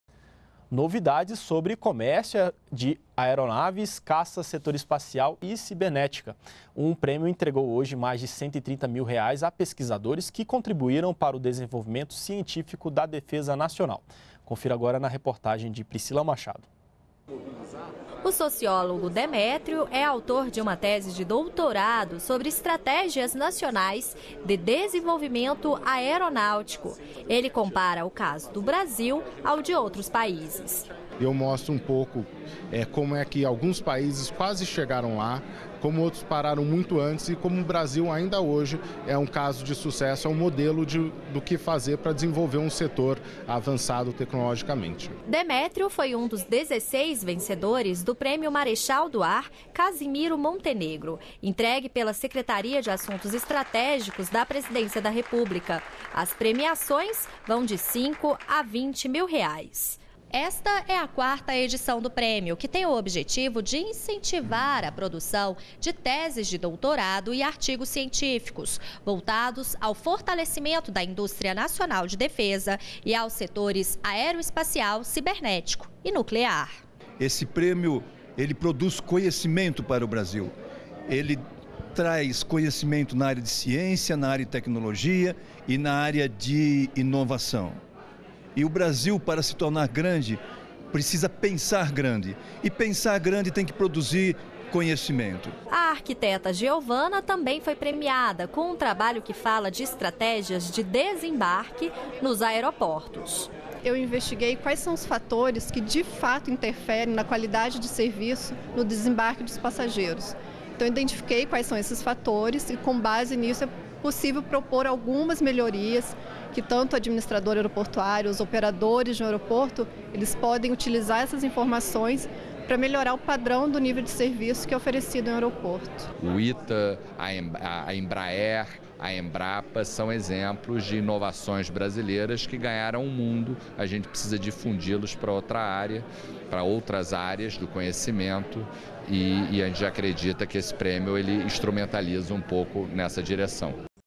Premiadas pesquisas sobre fortalecimento da Indústria de Defesa e dos setores aeroespacial
NBR NOTÍCIAS - 26.11.13: Nesta terça-feira (26), um prêmio entregou mais de R$ 130 mil a dezesseis pesquisadores que contribuíram para o desenvolvimento científico da Defesa Nacional. O Prêmio Marechal do ar Casimiro Montenegro Filho tem o objetivo de estimular a produção de estudos e pesquisas sobre o fortalecimento da Indústria Nacional de Defesa e dos setores aeroespacial, cibernético e nuclear.